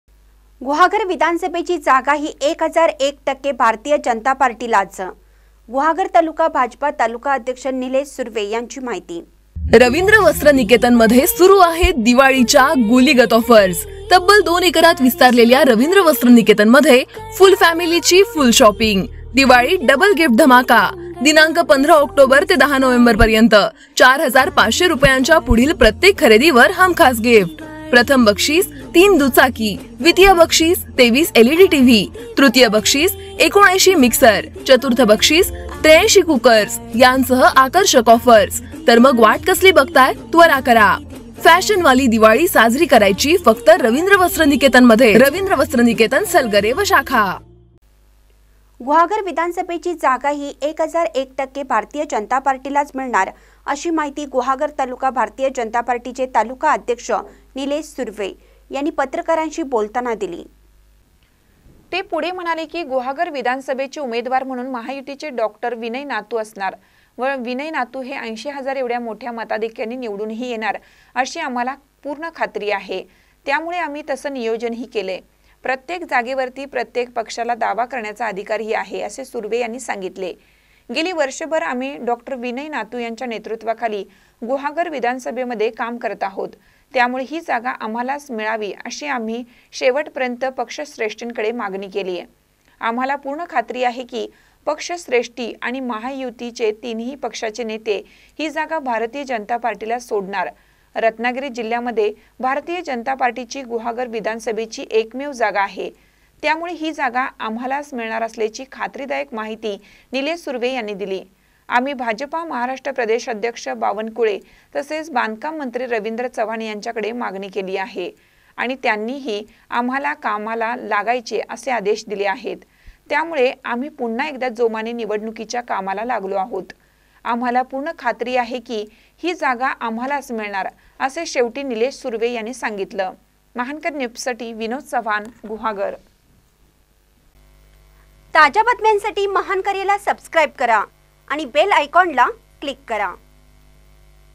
गुहागर विधानसभेची जागा ही 1001% भारतीय जनता पार्टीलाच - निलेश सुर्वे यांची माहिती
गुहागर विधानसभेची जागा ही 1001% भारतीय जनता पार्टीलाच,गुहागर तालुका भाजपा तालुकाध्यक्ष निलेश सुर्वे यांची माहिती